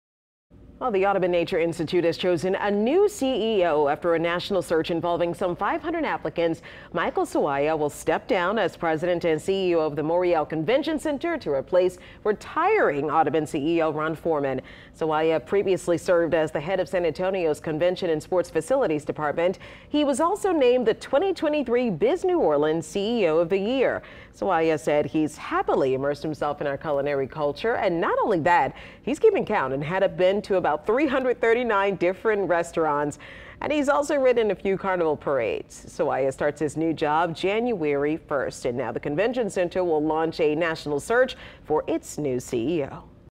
Audubon Nature Institute announces new CEO
Michael Sawaya will step down as President and CEO of the Morial Convention Center to replace retiring Audubon CEO, Ron Forman.